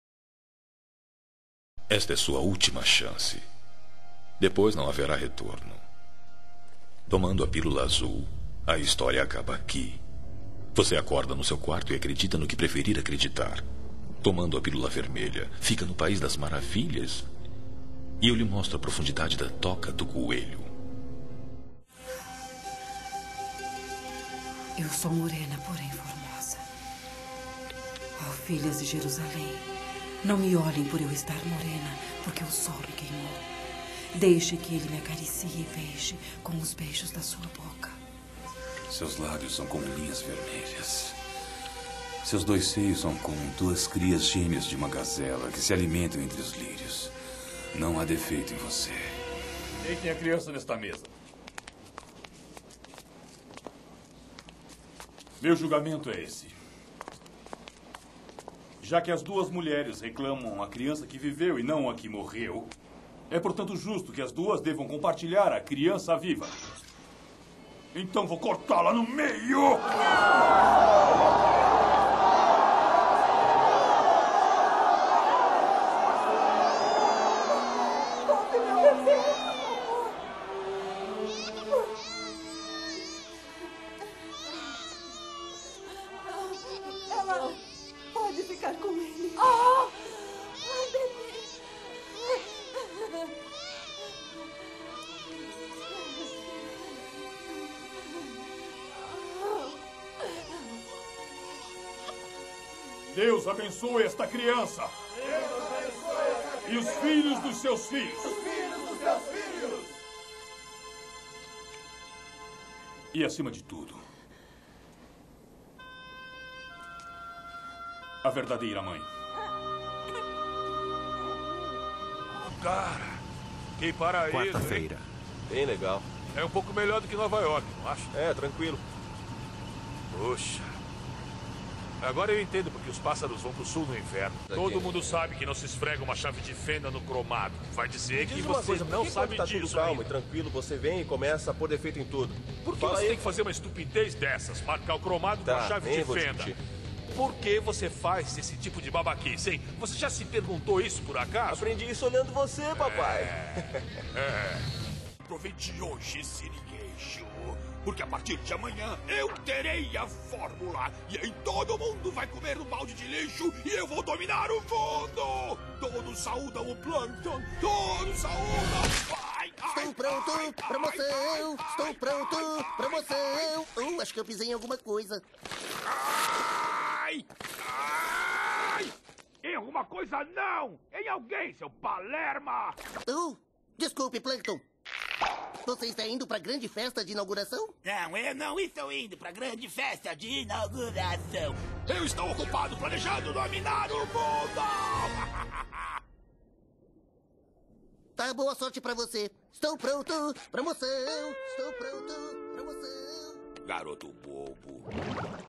Guilherme Lopes CLIP.WMV
Guilherme Lopes
Portfólio de alguns filmes com dublagem do ator